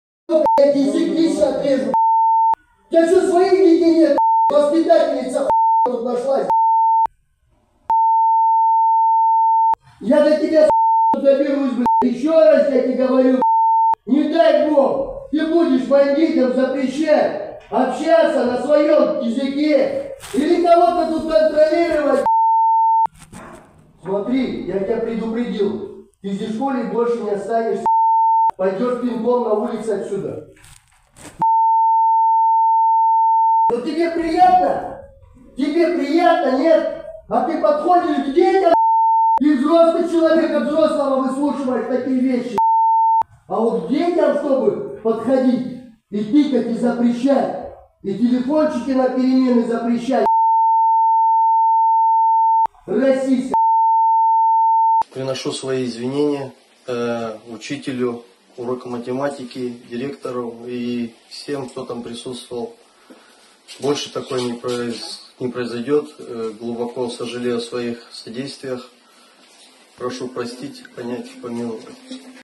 Отец школьника обматерил учителя в Челябинске
В Челябинске отец одного из учеников ворвался на урок с угрозами в адрес педагога. Крикливого папашу задержала полиция, он уже извинился. Ближайшие 15 суток он проведёт за решёткой. Силовики устанавливают причину конфликта